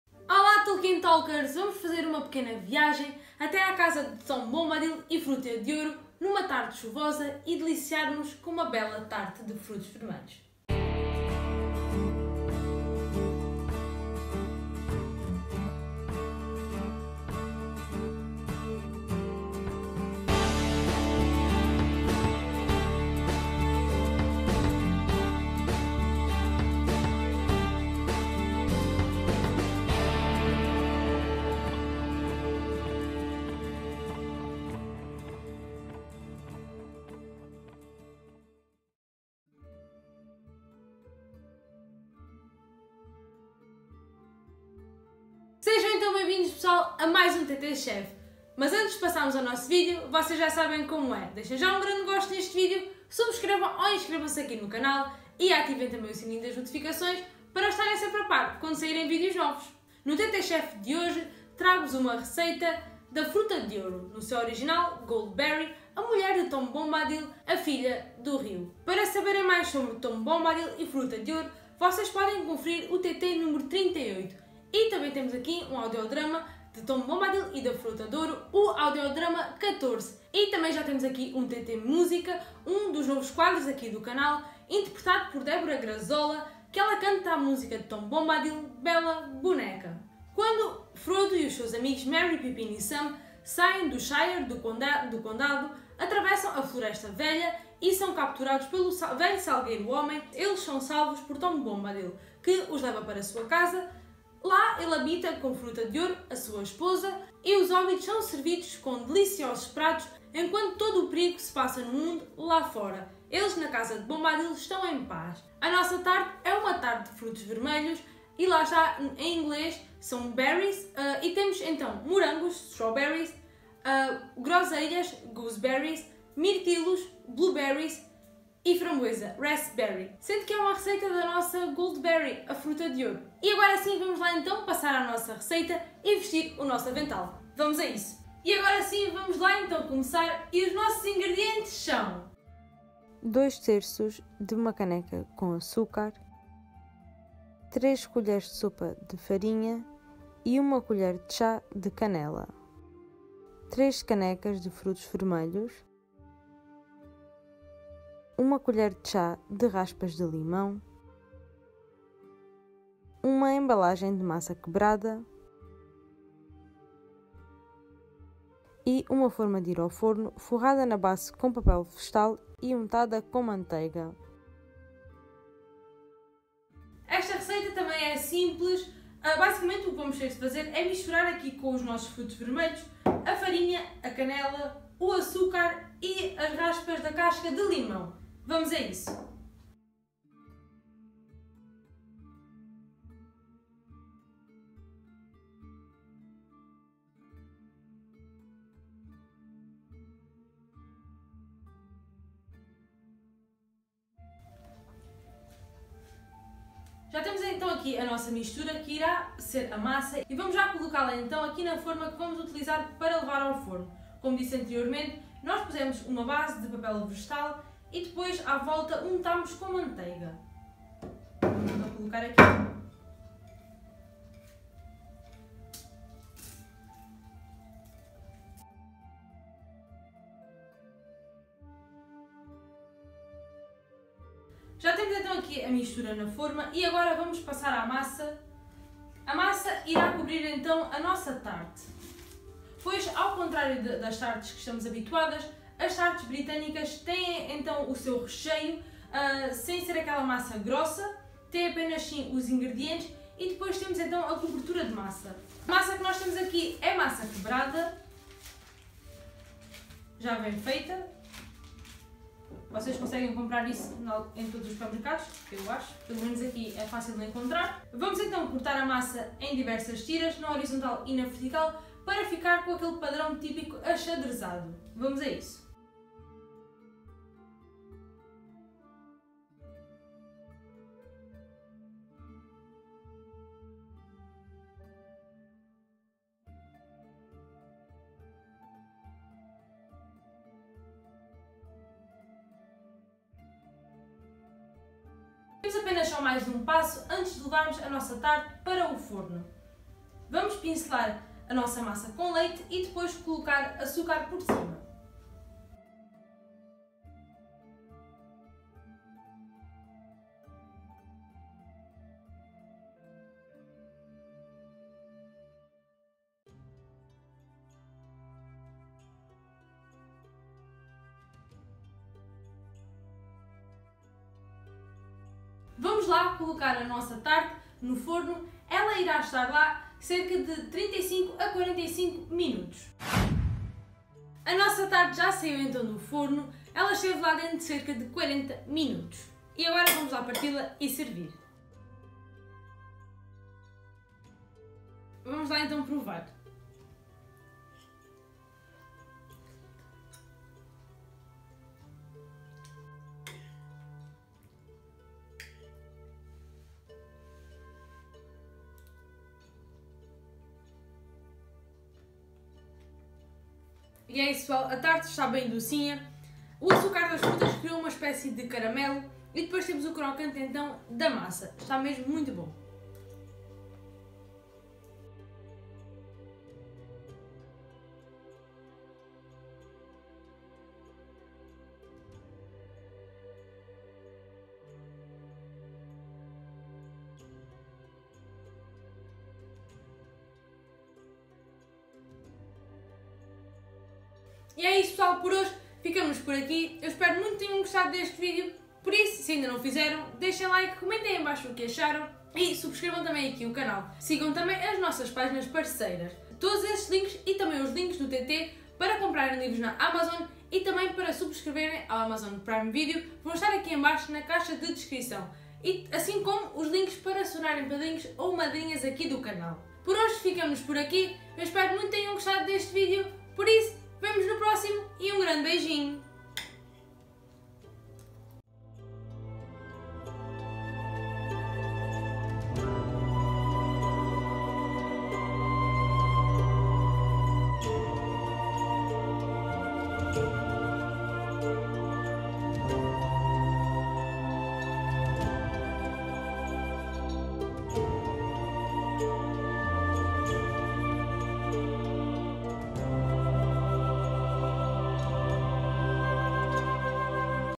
Cozinha de O Senhor dos Anéis: Tarte da Fruta d'Ouro | TT Chef 14
Há muitos mistérios que envolvem Fruta d’Ouro e Tom Bombadil mas os nossos hobbits tiveram a chance de visitar o seu lar e desfrutar da sua hospitalidade. Entre paz, descanso e longas histórias, foram servidos de maravilhosas refeições. No TT Chef de hoje, trazemo-vos a Tarte da Fruta D’Ouro com toda a doçura dos frutos vermelhos.